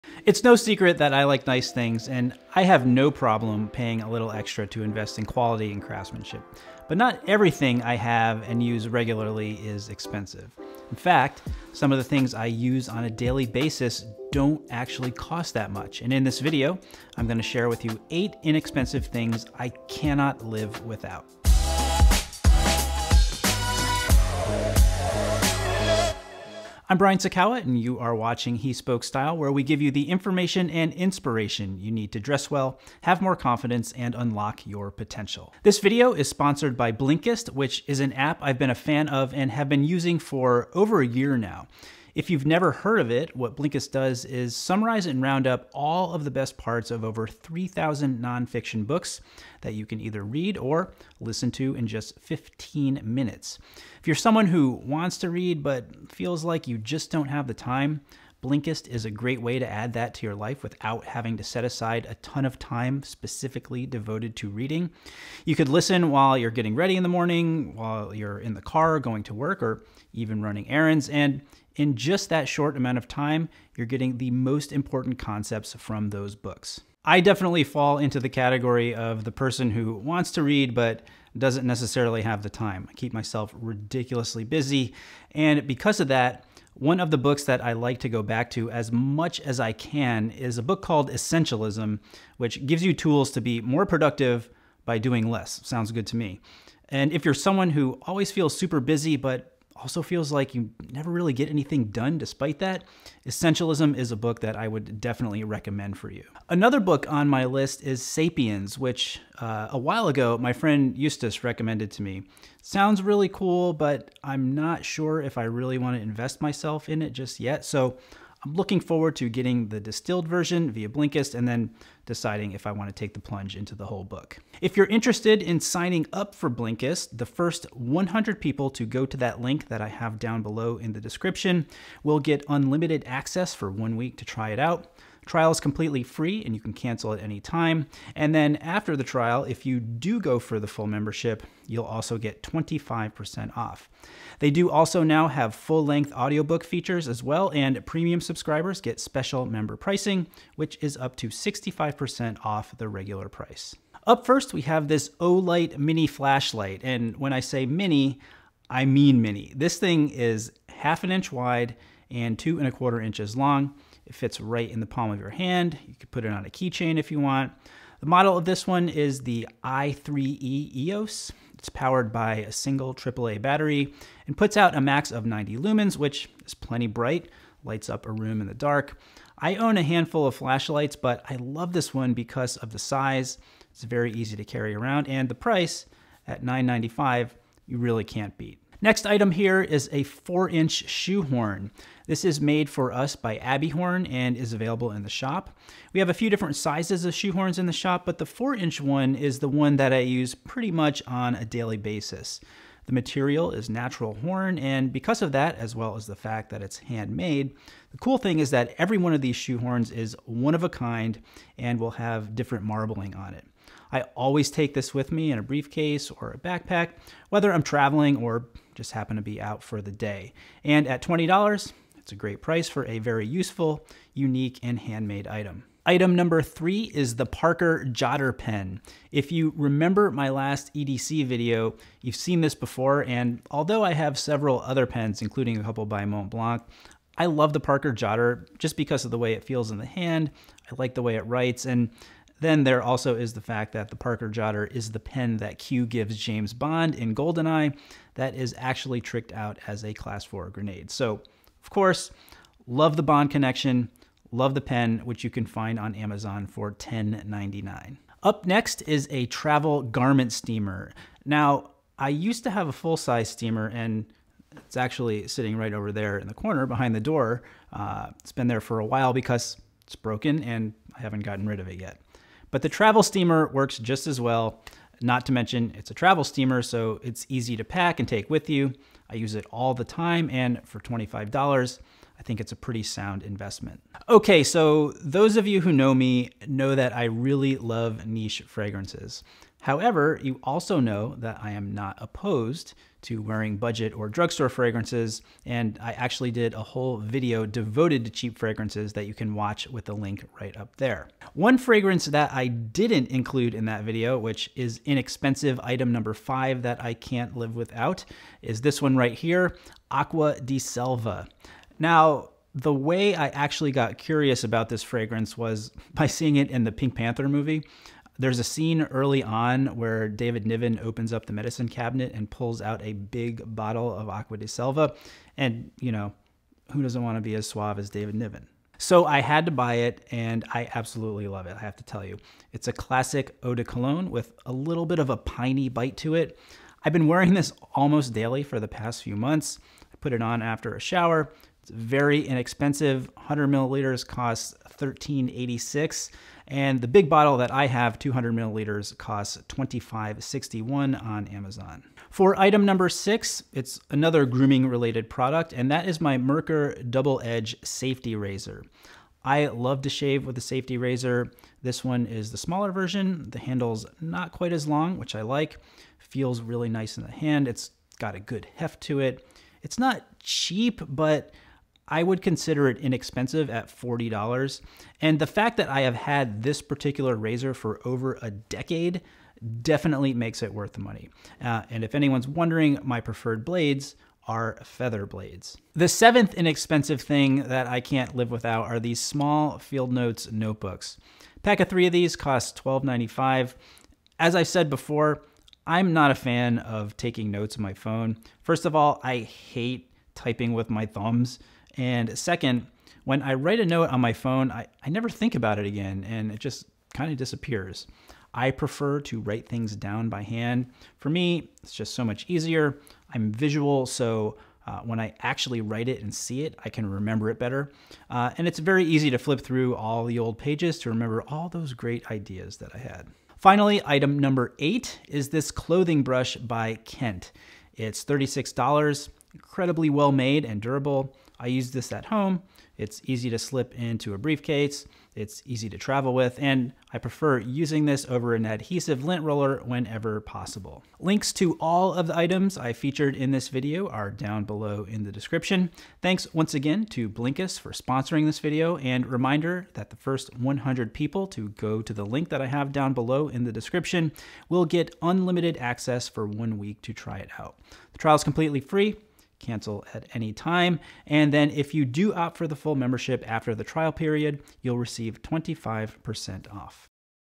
8 Inexpensive Things I Can't Live Without | Favorite Stuff & Gadgets 2021
are going to get unlimited access for 1 week to try it out.

It’s no secret that I like nice things and I have no problem paying a little extra to invest in quality and craftsmanship. But not everything I have and use regularly is expensive. In fact, some of the things I use on a daily basis don’t cost that much. And in this video, I’m going to share with you eight inexpensive things I cannot live without.